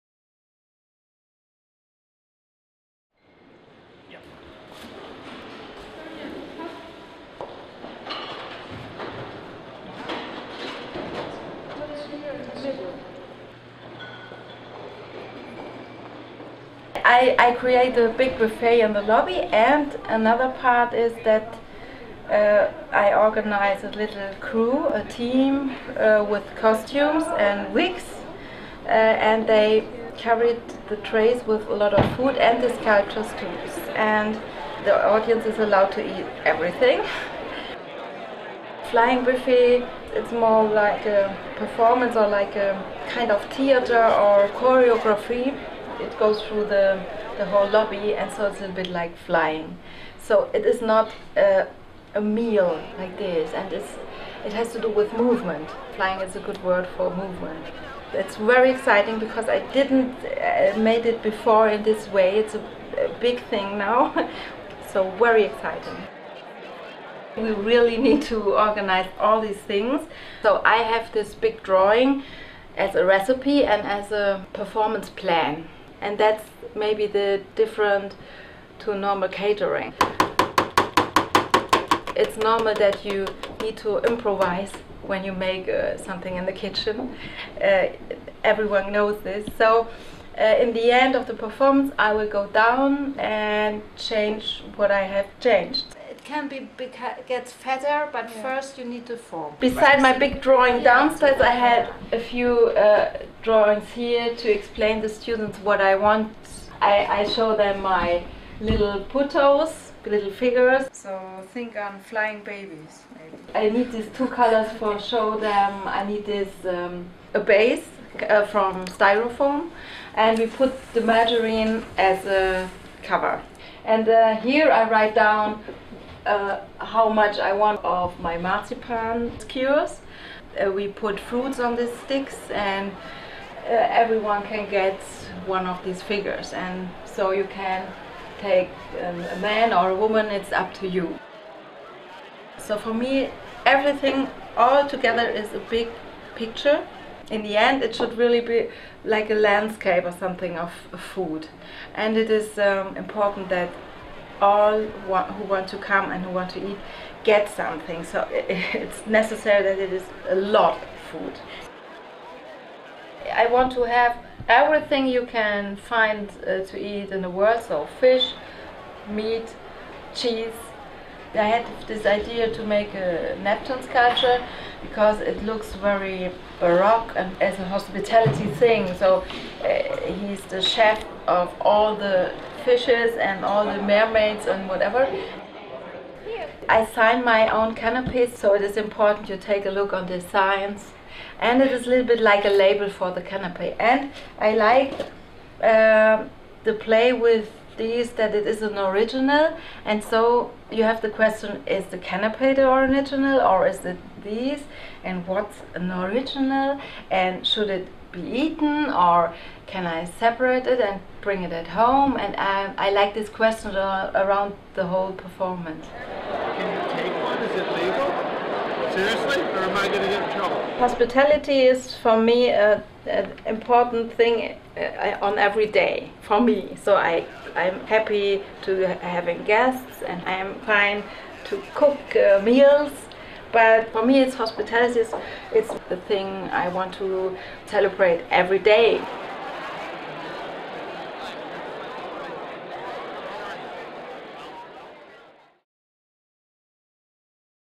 Sonja Alhäuser: Flying Buffet (Part of FEAST Fest)
Artist Sonja Alhauser talks about using a recipe drawing to create a "catering performance" for Feast.